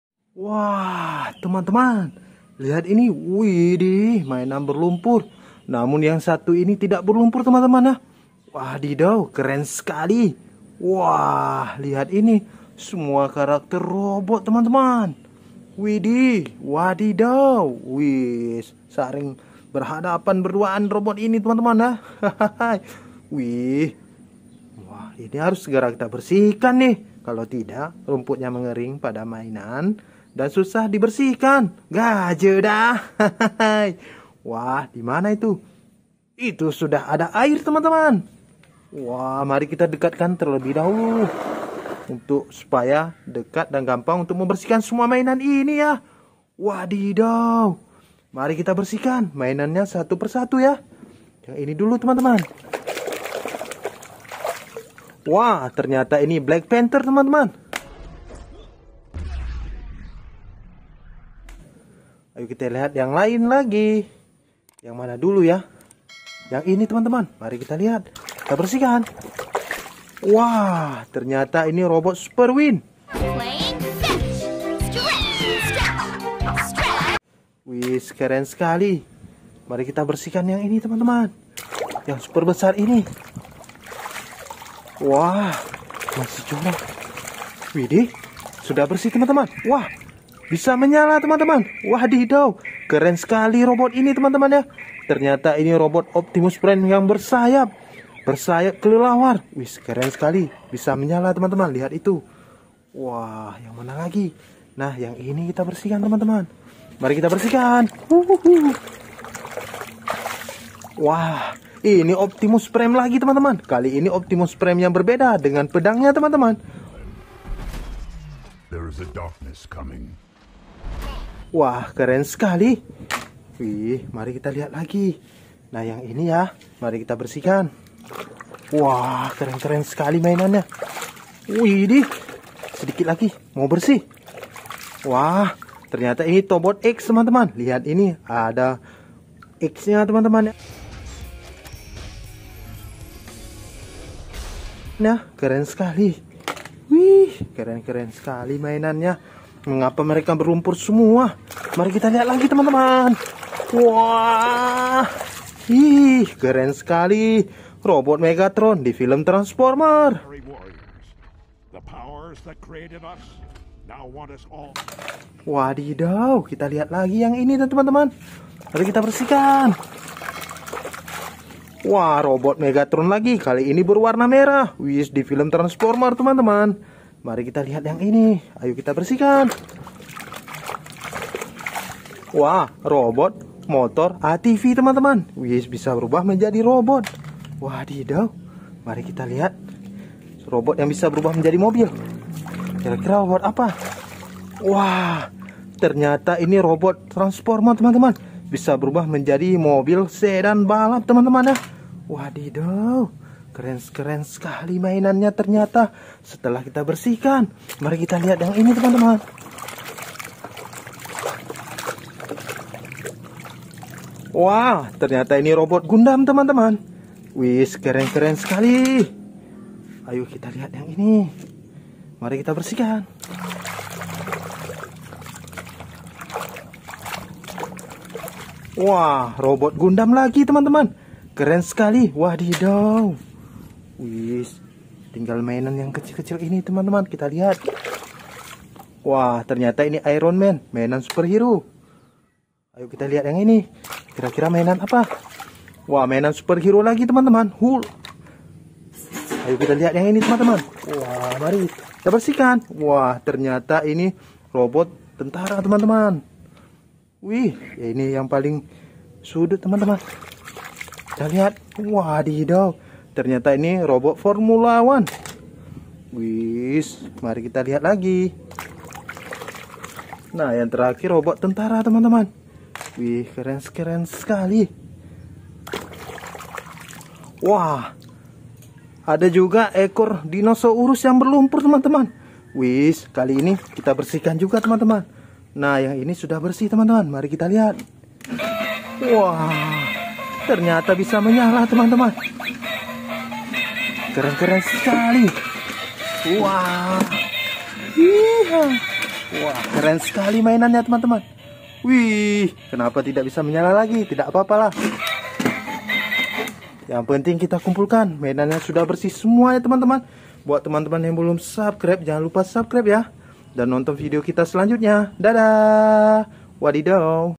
Membersihkan Mainan, Robot, Optimus Prime, Bumblebe, Tobot X, Robot Gundam, Super Wings
Di video kali ini kita akan Membersihkan Mainan, Robot, Optimus Prime, Bumblebe, Tobot X, Robot Gundam, Super Wings